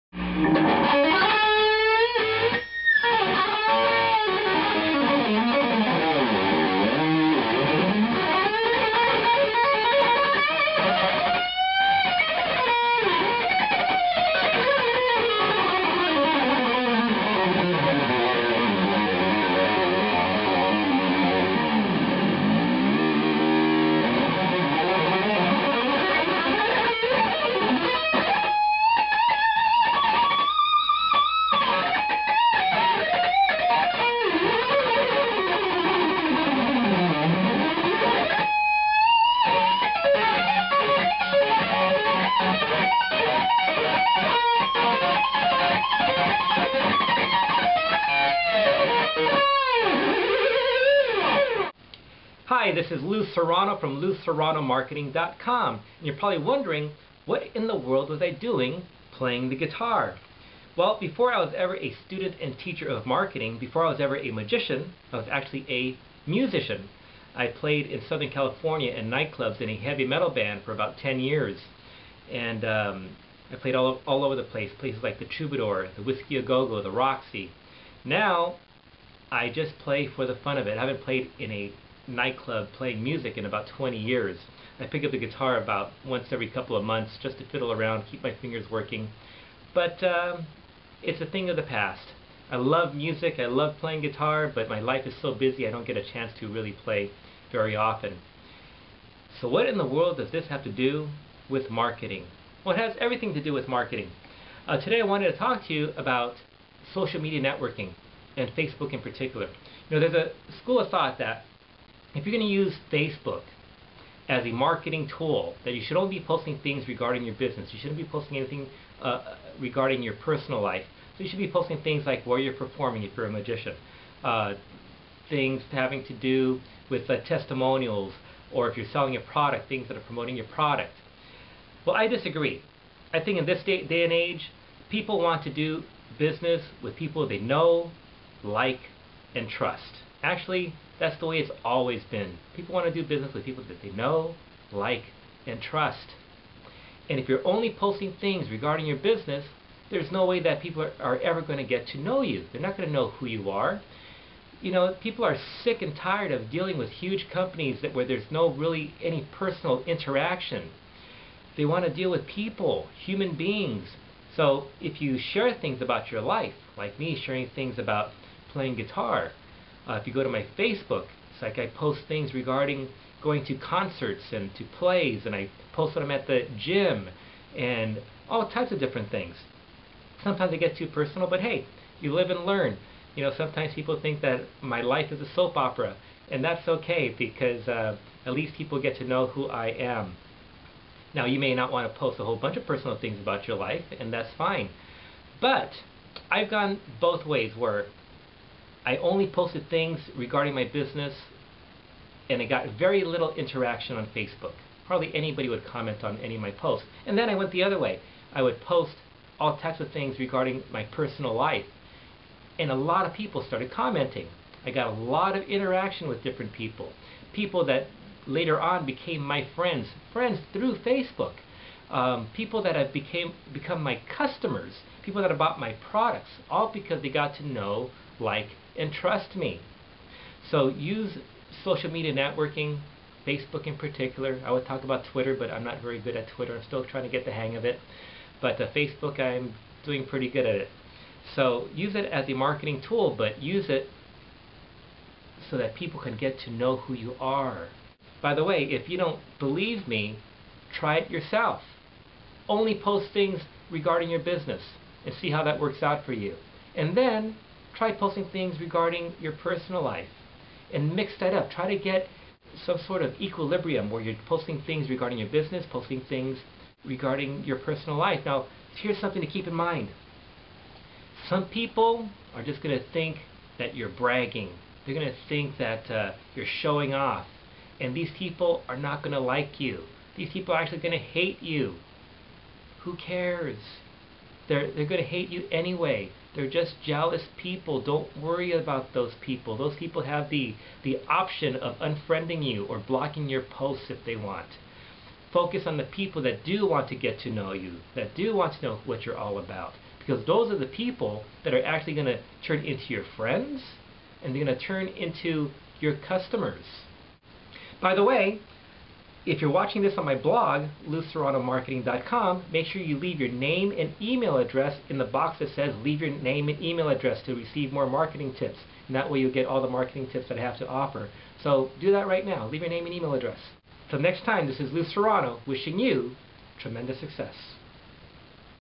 Social Media & Facebook: How to maximize your efforts.
Lou Serrano talks about the correlation between playing guitar and effective marketing through social media and Facebook.